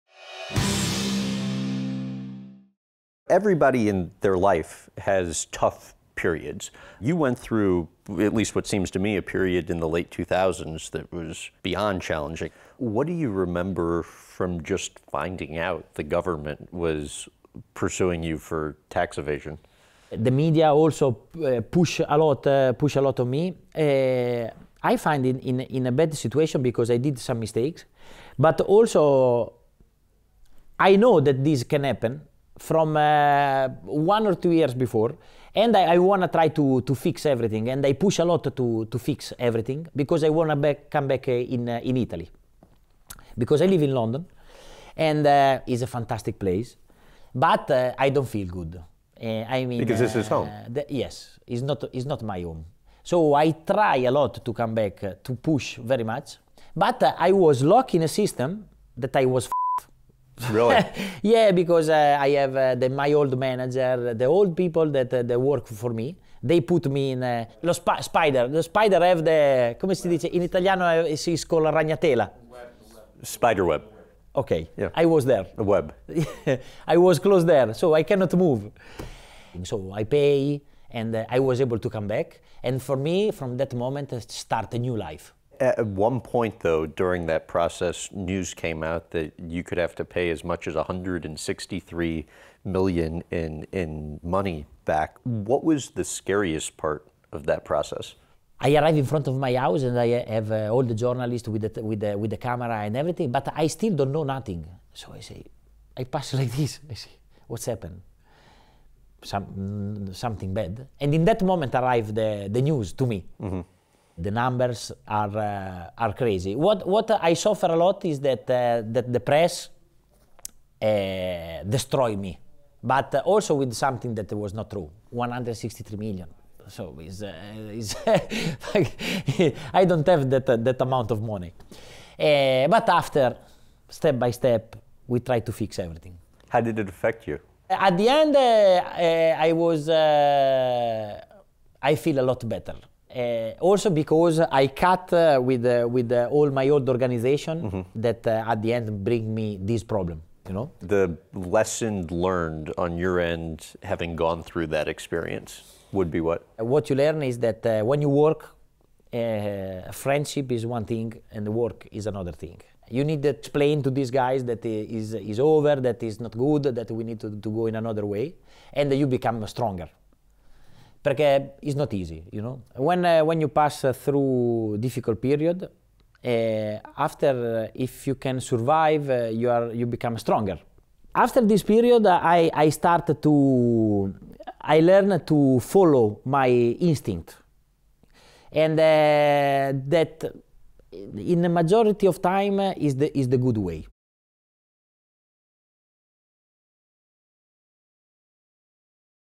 Valentino Rossi on $163 million tax evasion claim
MotoGP legend Valentino Rossi recalls first learning the Italian government claimed he owed roughly $163 million in back taxes. The investigation was linked to Rossi's decision to move to London in 2000. Rossi explains how he worked to resolve the claim and how the hardship showed him the difficulties of mixing business with friendships.

IN ITALIANO: La leggenda della MotoGP Valentino Rossi ricorda di quando apprese che per il governo italiano era debitore di circa 163 milioni di dollari per tasse arretrate. L'indagine era collegata alla decisione di Rossi, nel 2000, di stabilirsi a Londra. Il pilota spiega in che modo risolse il problema e come quella vicenda gli abbia mostrato la difficoltà di conciliare affari e amicizie.